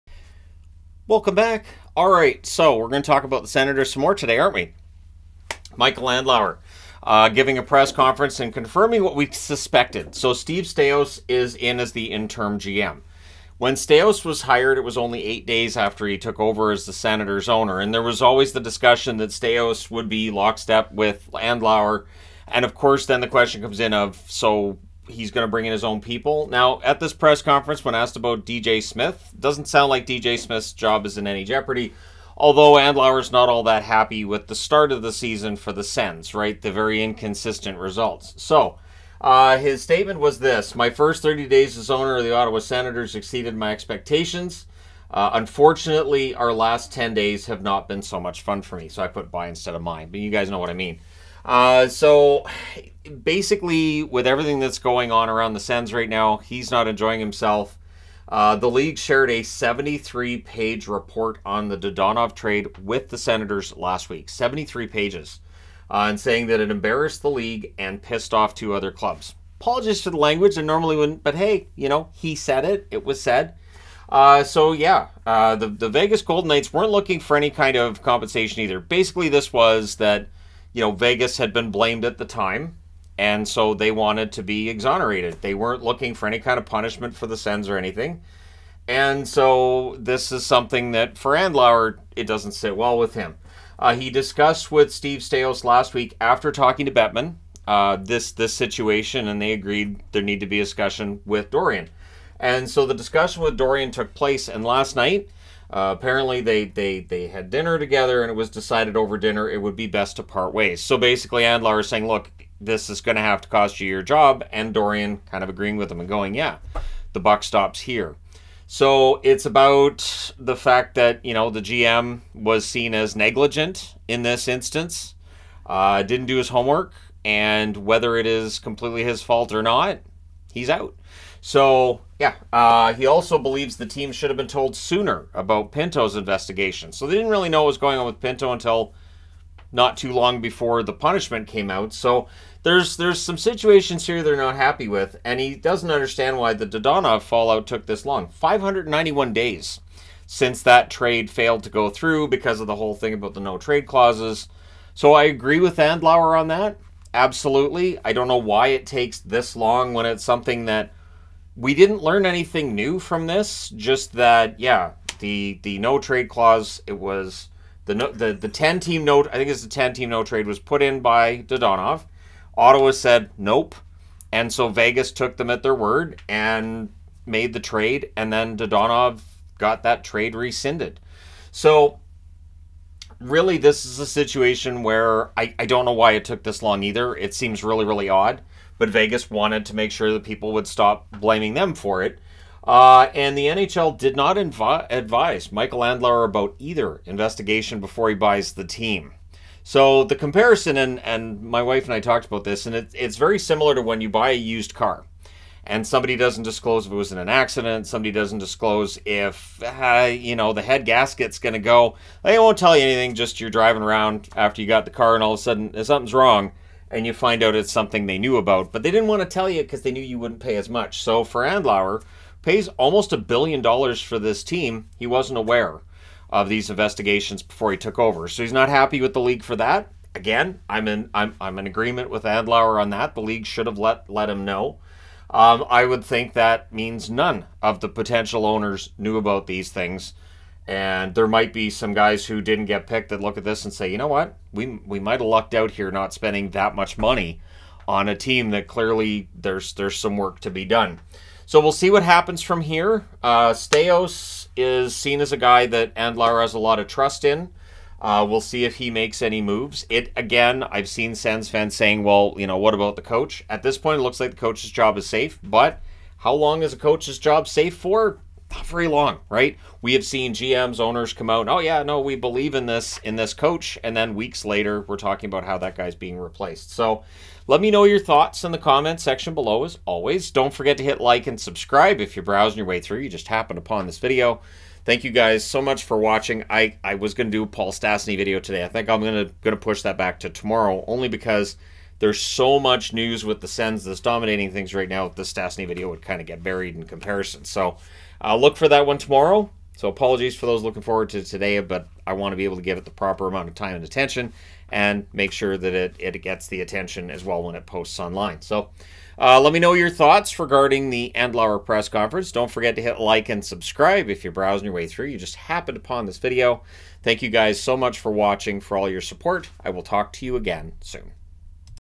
Andlauer Installs Staios as Interim GM, Unhappy with Recent Events
Well, here's another video on the Ottawa situation.

Abbotsford, BC
V2S 8P1

In the USA:
Shannon Skanes
1125 Fir Avenue
Suite 119